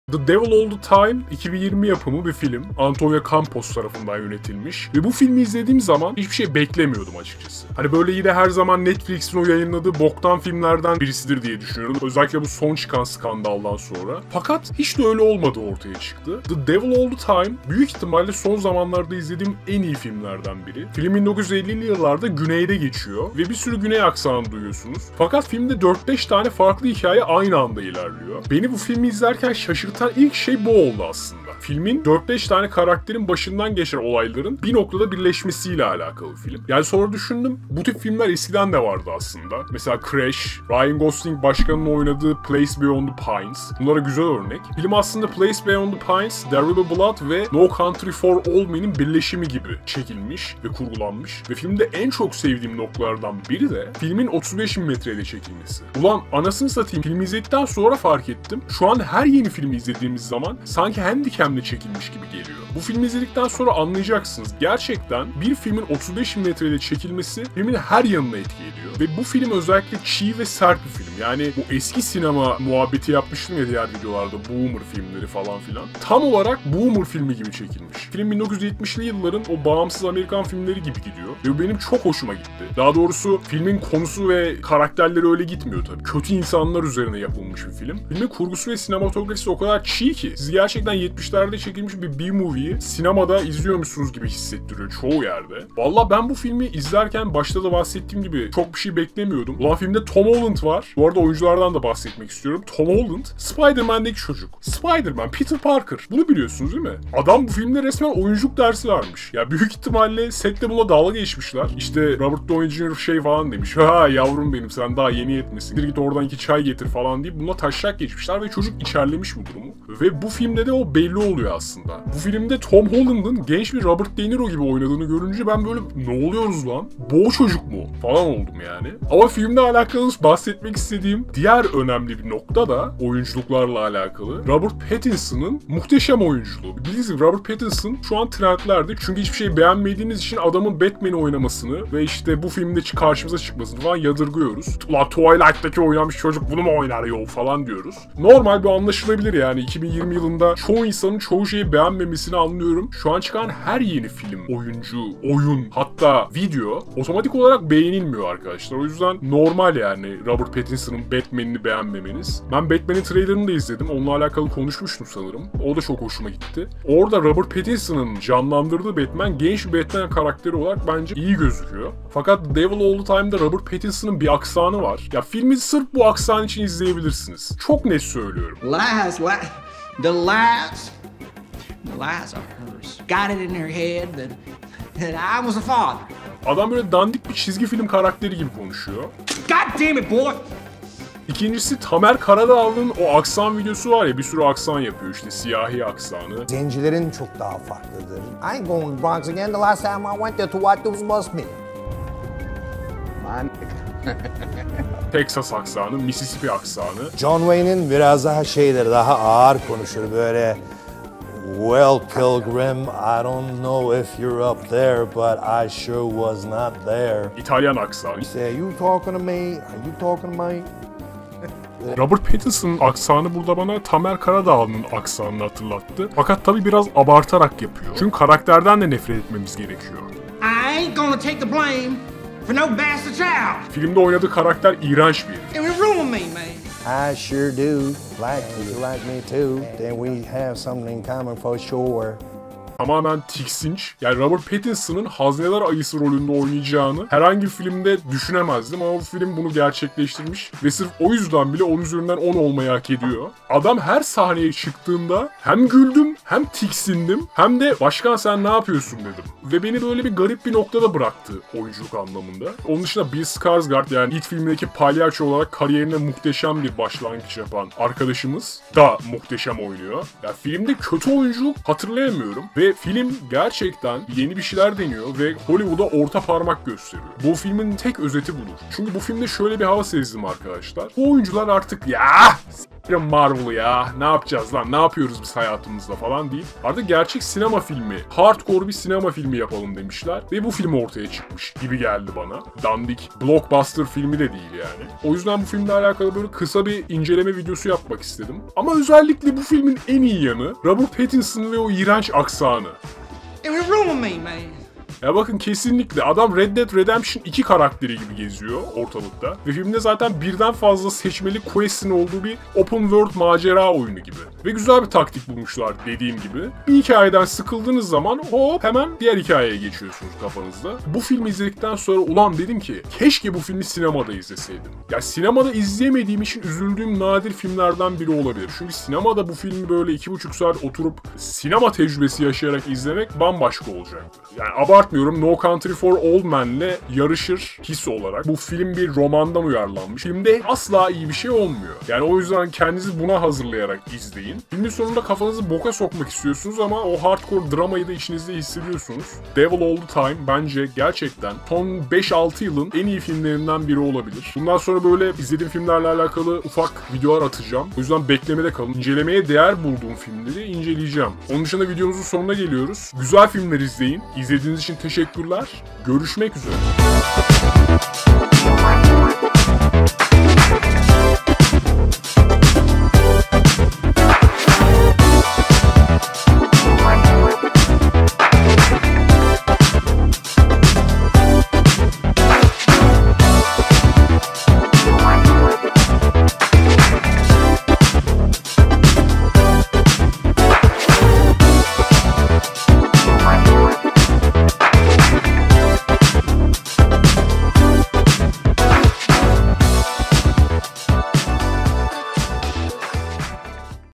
SONUNDA İYİ BİR FİLM GELDİ
Devil All The Time (2020) baya Kino bir film. Özellikle Robert Pattinson'un aksanı muhteşem. 10/10.